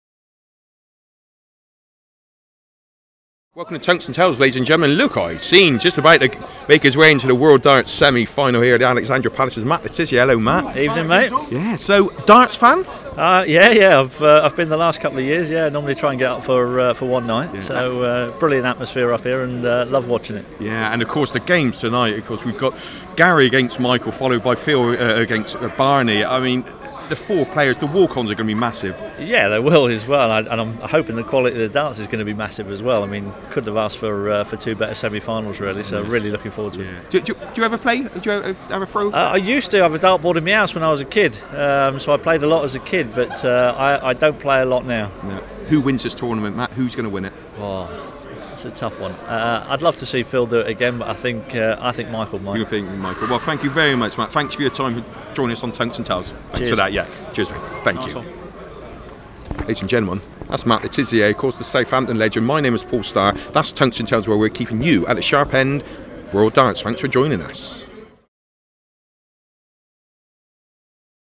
Matt Le Tissier talks to Tungsten Tales at the Darts World Championship
Who does he think will be crowned champion?